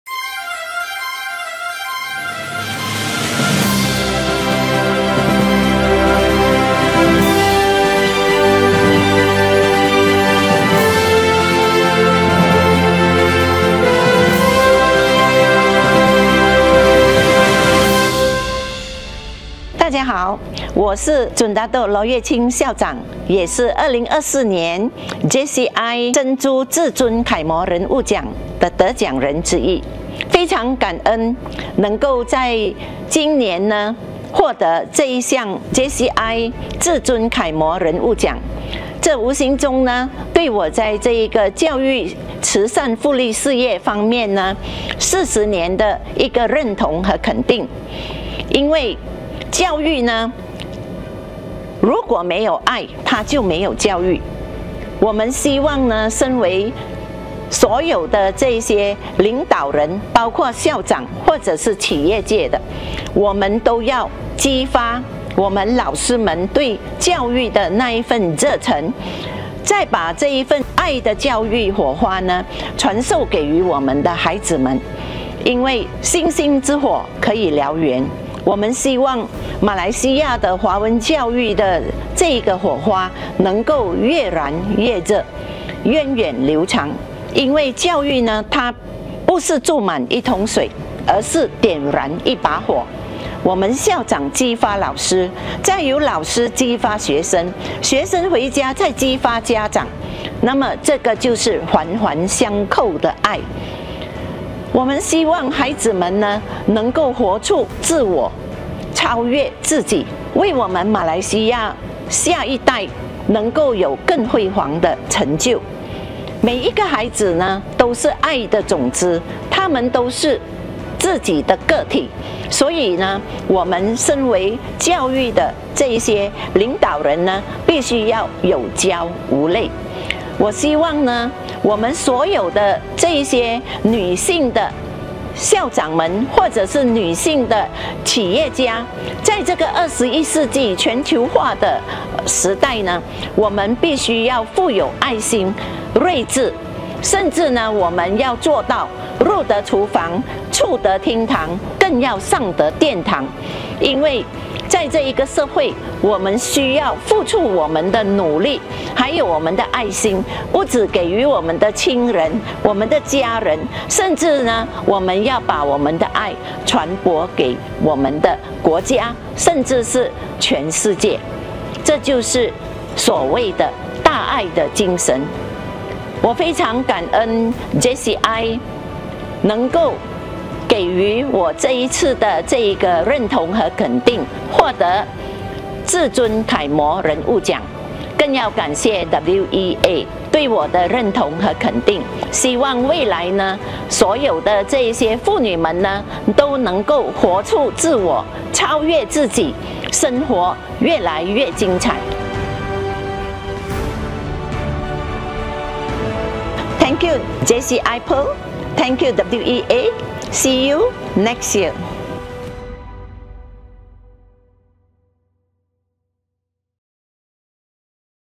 Pearl Role Model Award 至尊楷模人物 - LOH YAT CHENG, DJN, AMN, PKT, PJK
2024 Pearl Women Entrepreneur Award (WEA)

Pearl Women Entrepreneur Awards aims to recognize female entrepreneurs with outstanding career achievements and contribution to the society development as well as to encourage more young women to keep entrepreneurial spirit alive.